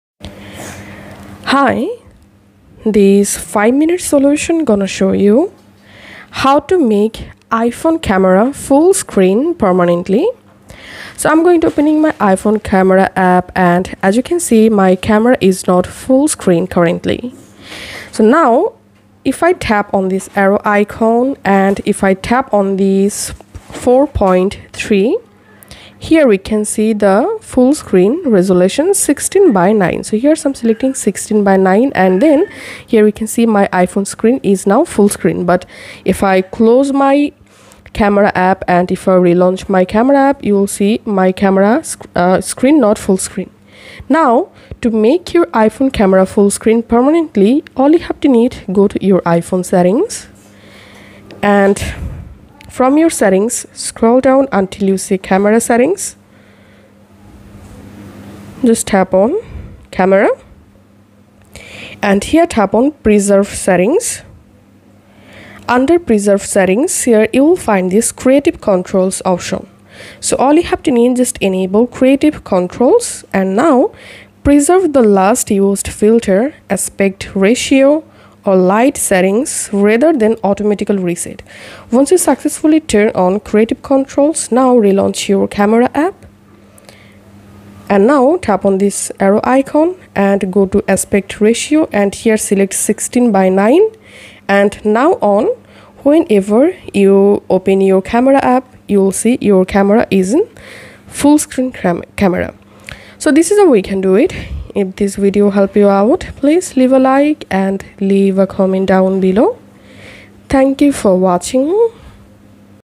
How To Make iPhone Camera Fullscreen Permanently - Full Guide
Interested in seeing your iPhone in full screen, with no menu bar? There isn't a system-wide full-screen mode option, here we can provide Make iPhone Camera Fullscreen Permanently